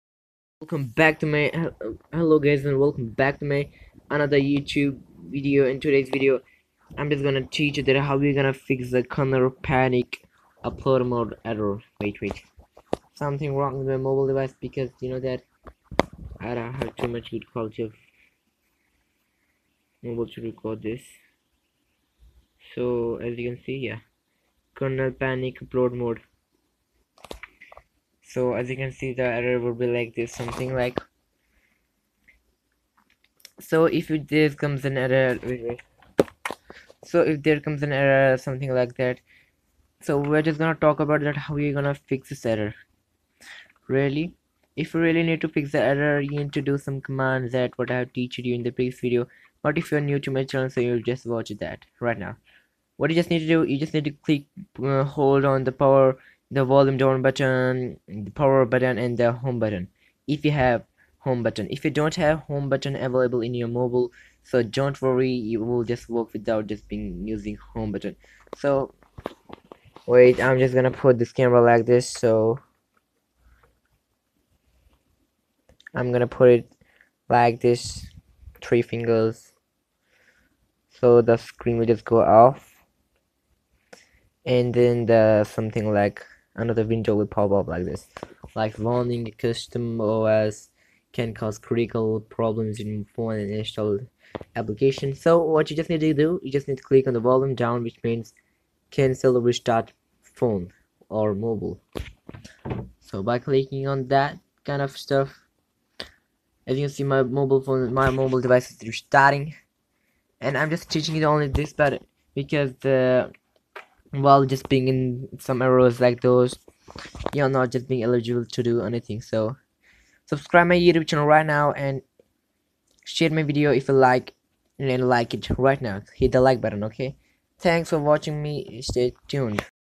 kernal panic upload mode
it is so easy to fix kernal panic upload only you need to press 2 buttons and the problem would be resolved.